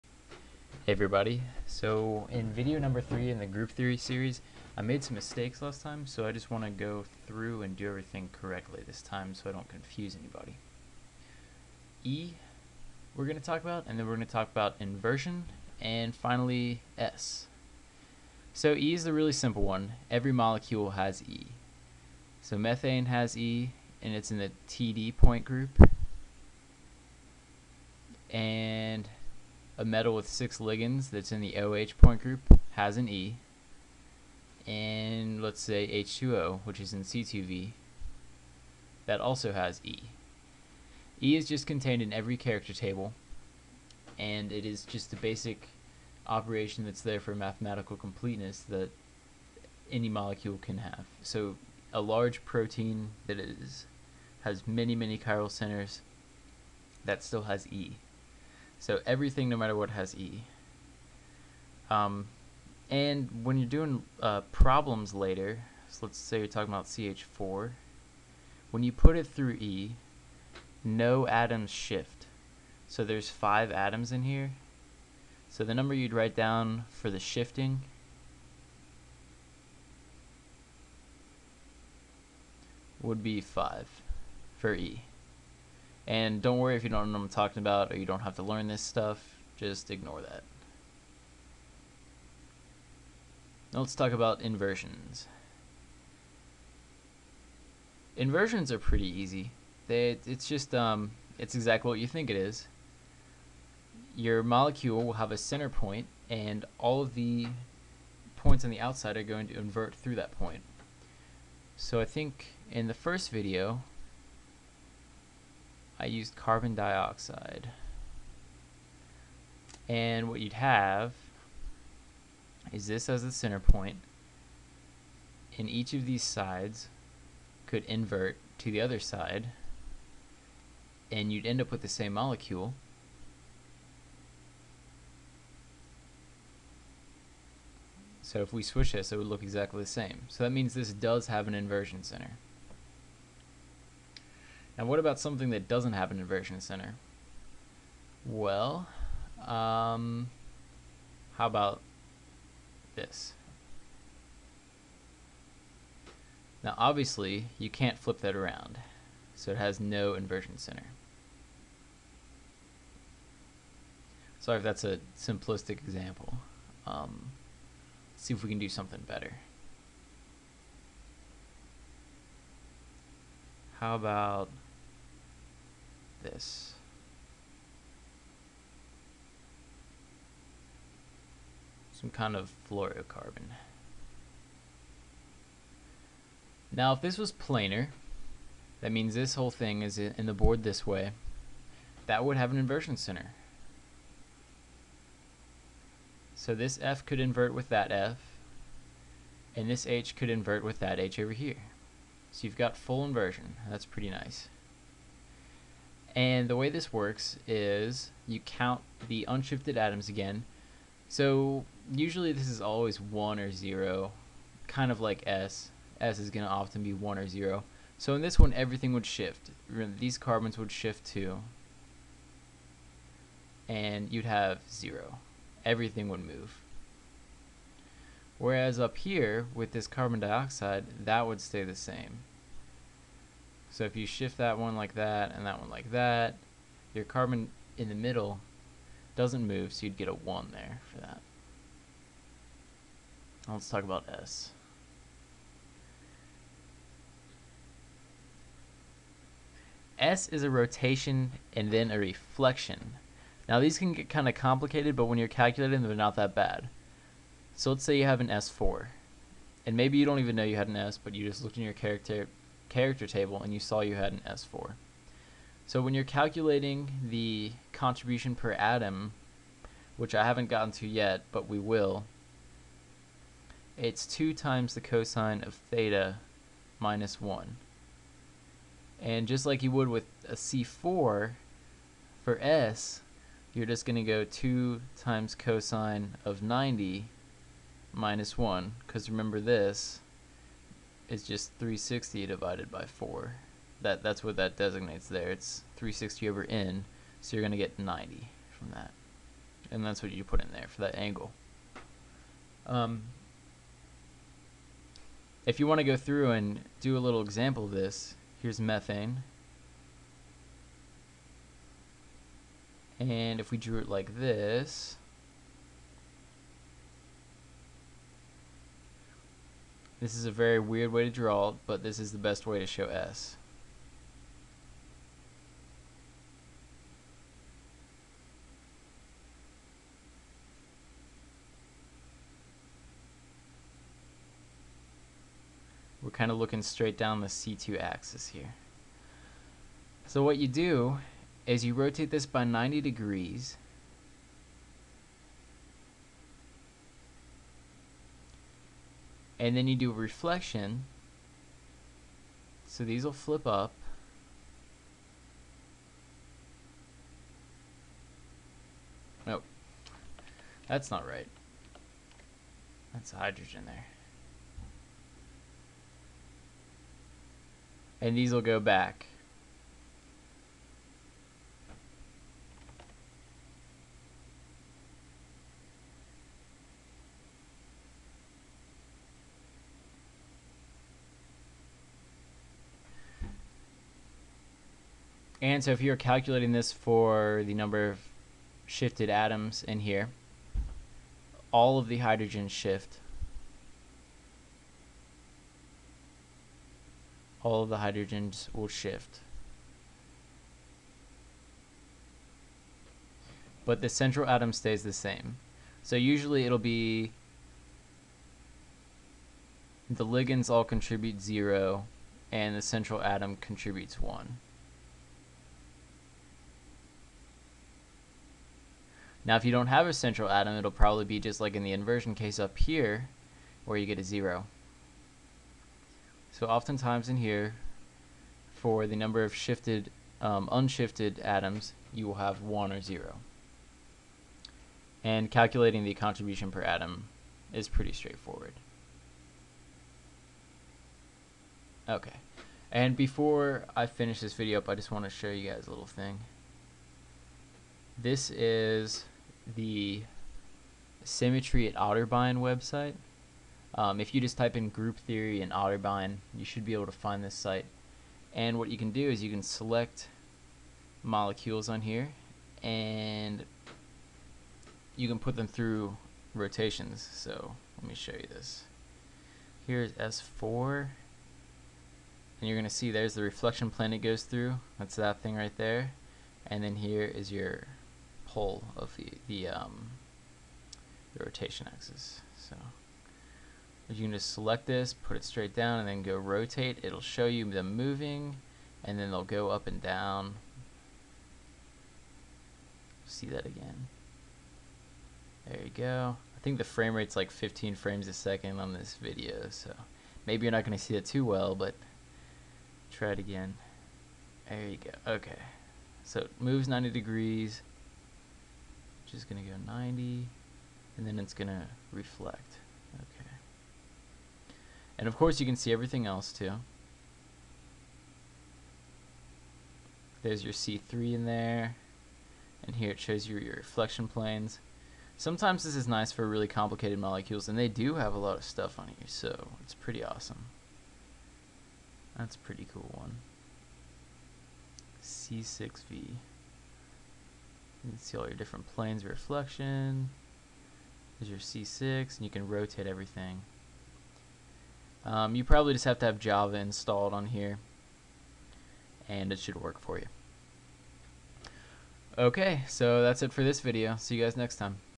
Group Theory Part 3: Symmetry operations E, Inversion, and S (redo)
I go over E, I, and S operations using Ferrocene, Carbon Dioxide, Methane, and others as examples